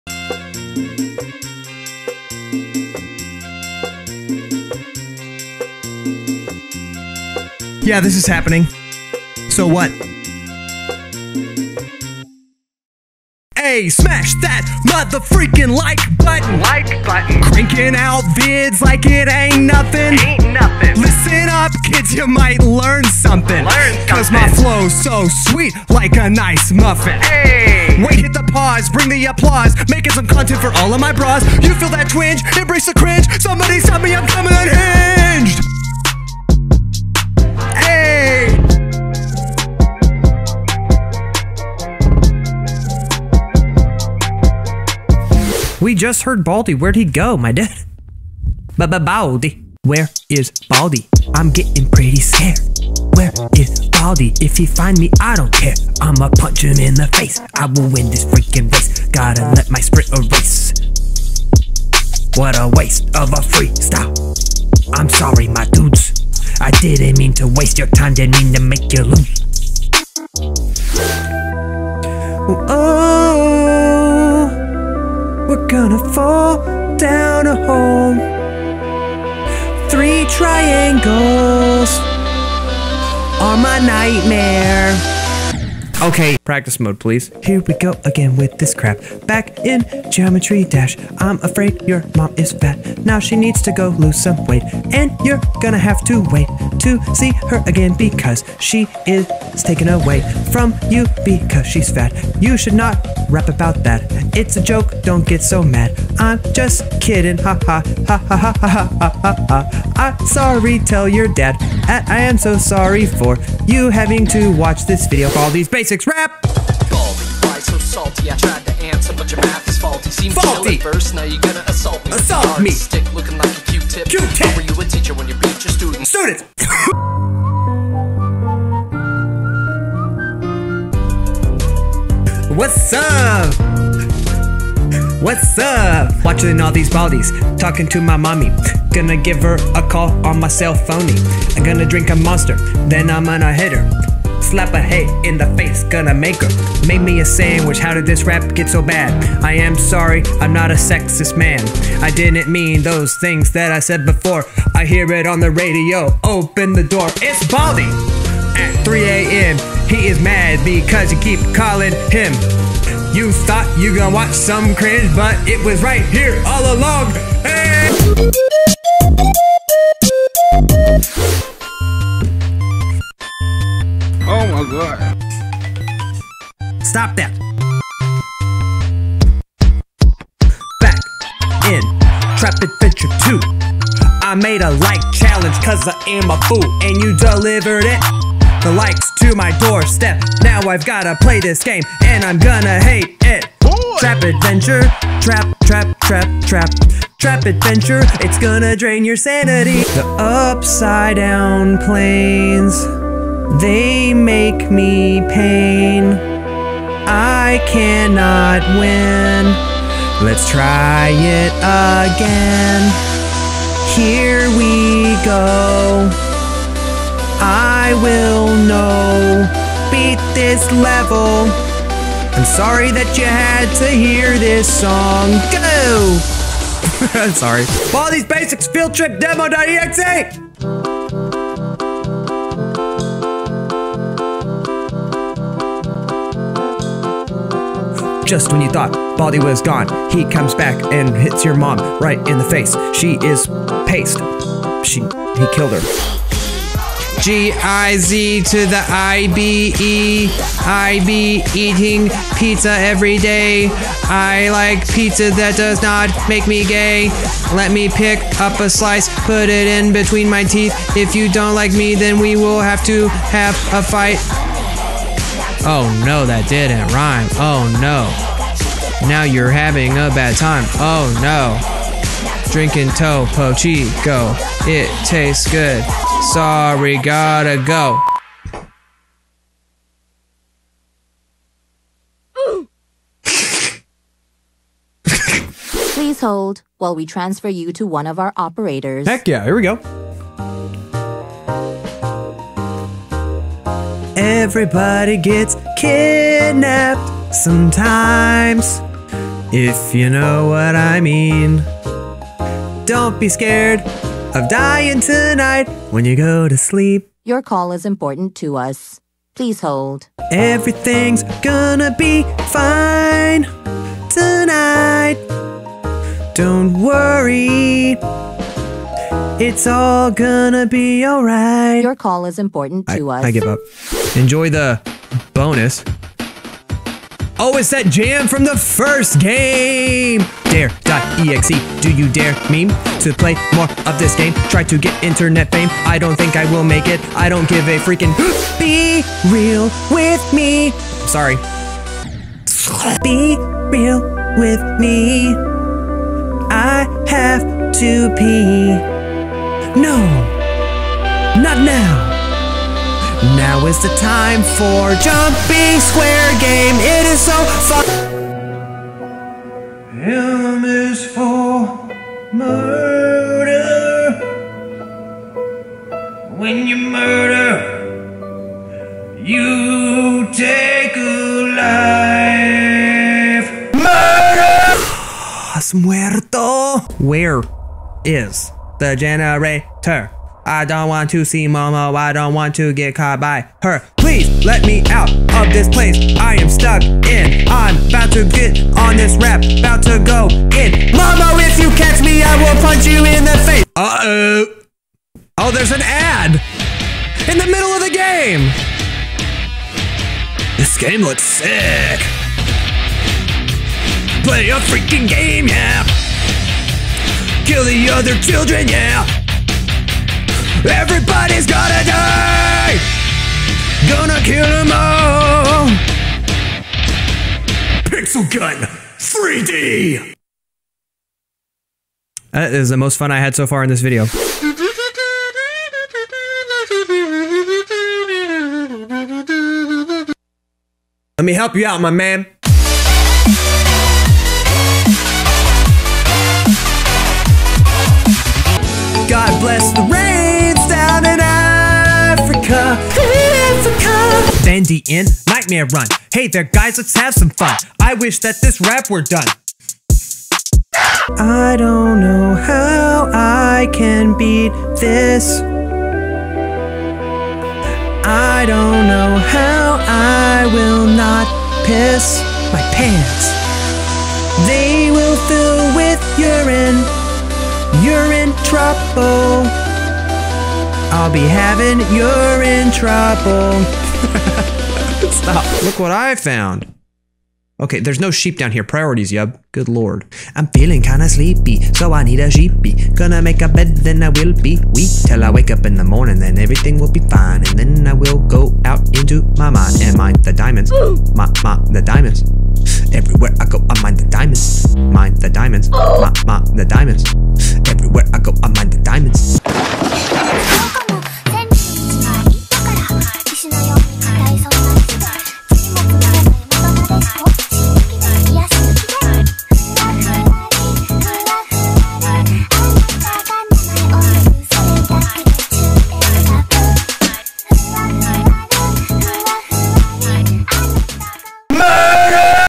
YUB'S GREATEST HITS #5 - Stupid Gaming Raps & Songs Montage
All my videos used in this gaming rap compilation are linked below, click "show more"!

HERE ARE ALL THE VIDEOS I USED:
0:01
0:45
1:11
1:55
2:48
3:20
3:48
4:06
4:45
5:24
5:38
5:58
6:15
6:33
7:04
7:24
7:31
7:55